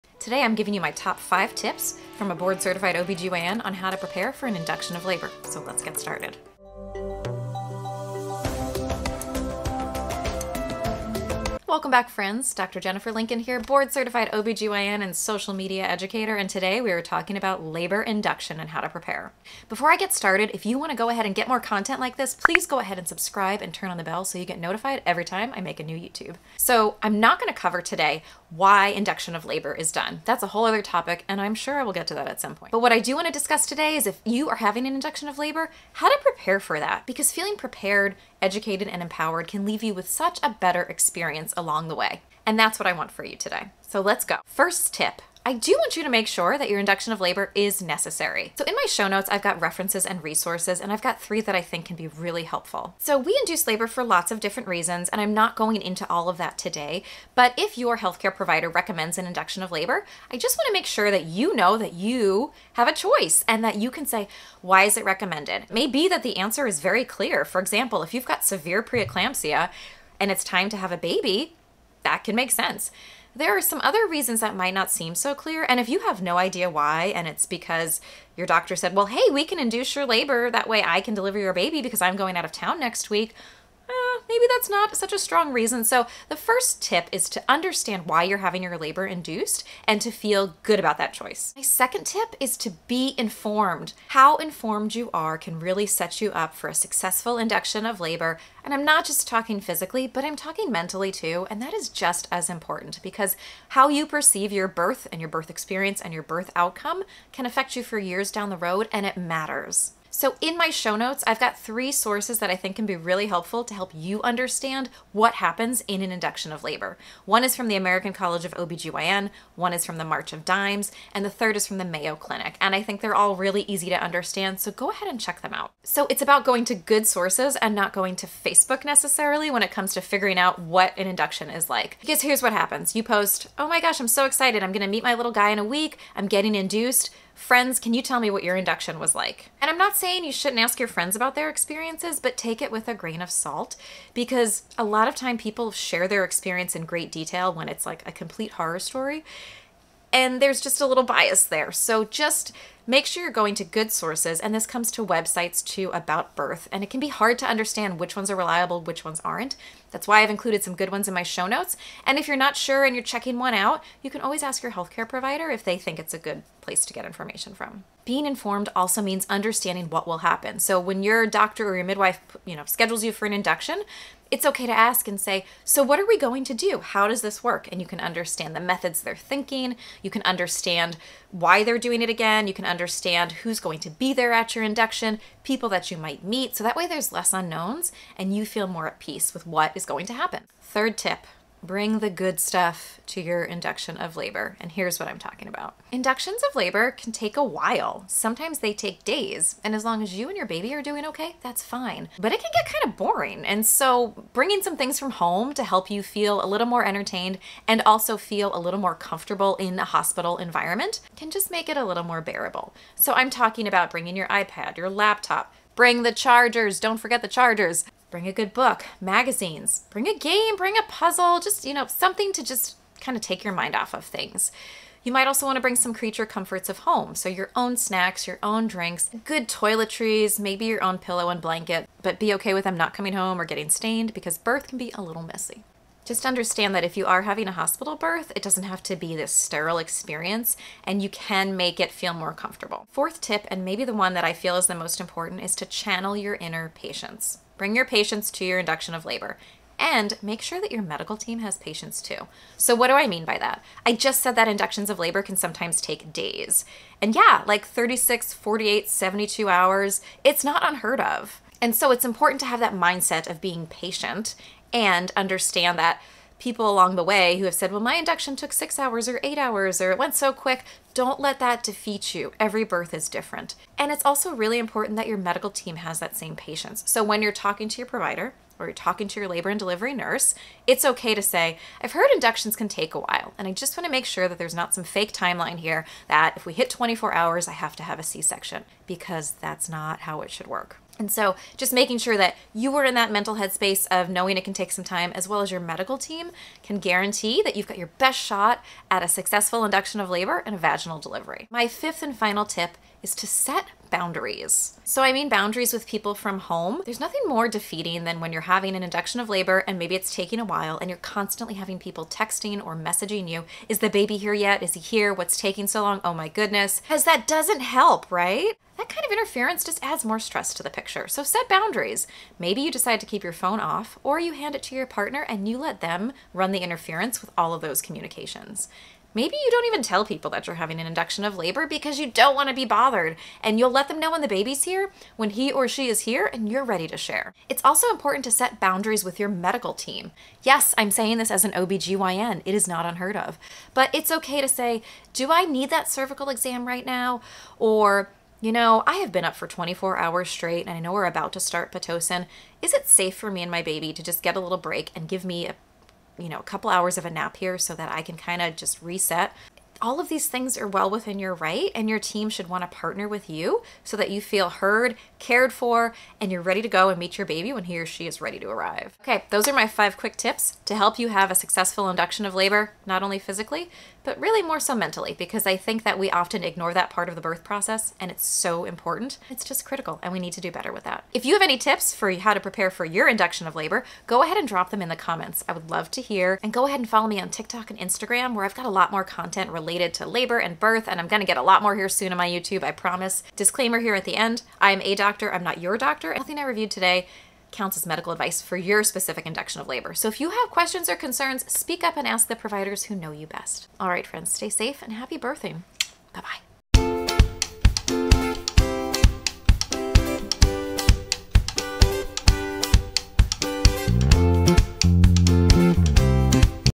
OBGYN tips for preparing for labor induction
Today Dr. Lincoln shares her top 5 tips for how to prepare for your induction of labor - and these are tips you don't want to go without. Feel empowered, educated, and prepared so you can meet your baby and have a positive experience!

0:00 Intro
0:44 Make sure your induction is necessary
1:31 Be informed
3:10 Bring things from home!
4:02 Be patient - you *and* your medical team
5:01 Set boundaries

REFERENCES and RESOURCES:

1. The American College of OBGYN. FAQ: Labor induction.

2. March of Dimes. Inducing labor.

3. Mayo Clinic. Labor induction.

BE SOCIAL!

Don't forget to come follow along elsewhere!

THE BEAUTIFUL THINGS

Artwork credits:
Vertical uterus
Horizontal uterus with flowers